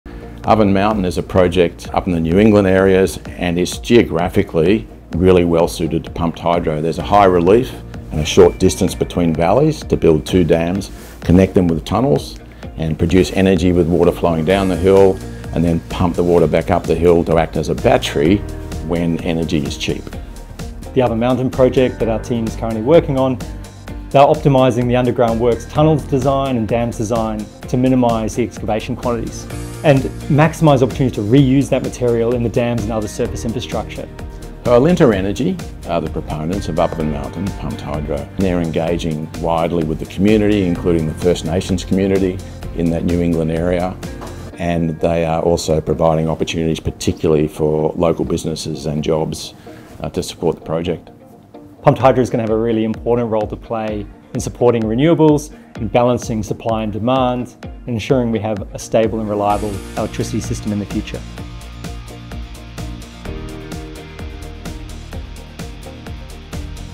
Oven Mountain Pumped Hydro
The 900MW Oven Mountain Pumped Hydro project is a key energy storage initiative in NSW’s New England Renewable Energy Zone. Using off-river reservoirs and a closed-loop system, it acts like a giant battery—pumping water uphill with renewable energy and releasing it on demand.